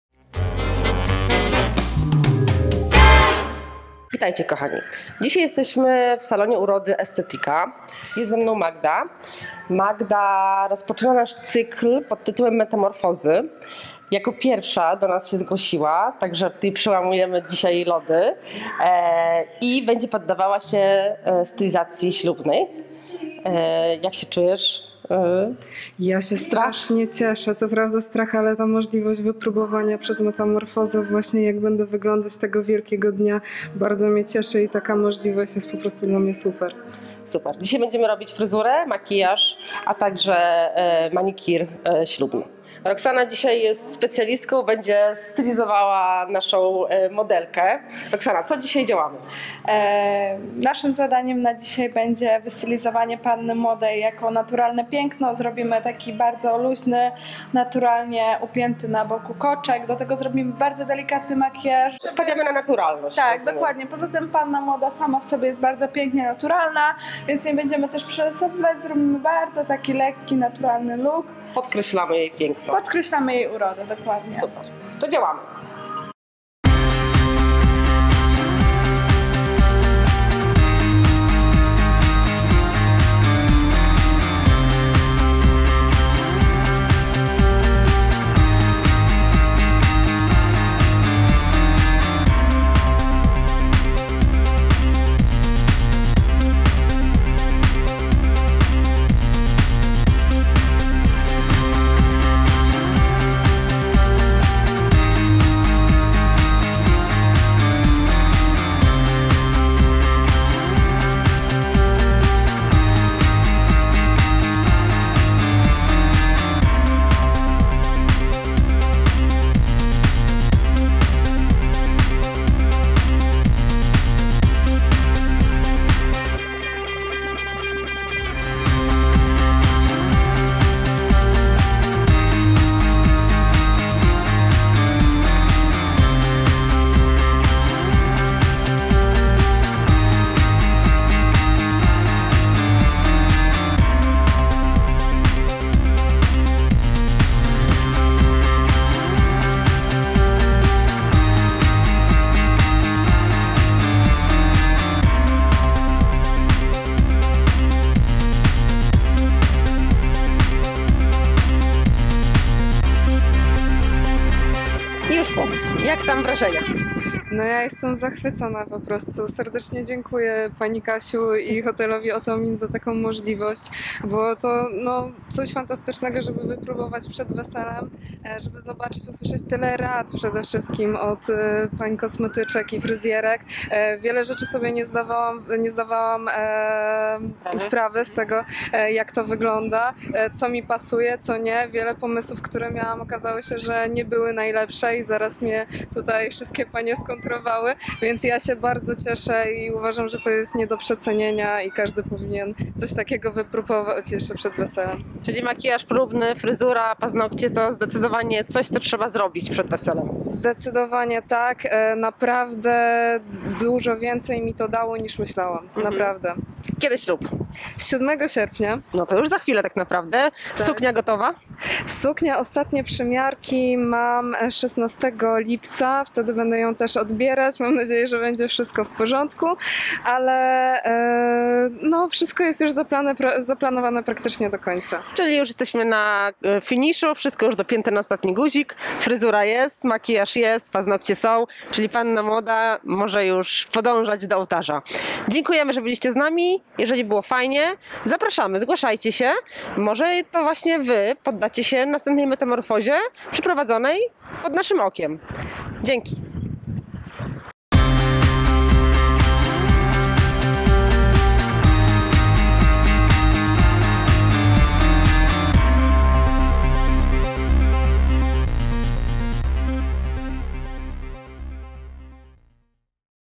metamorfoza
Przykładowy makijaż, upięcie oraz manicure ślubny! Zobaczcie jaką metamorfozę przeszła Magda!
Chcecie więcej porad i inspiracji? Zapraszam na mój blog ślubny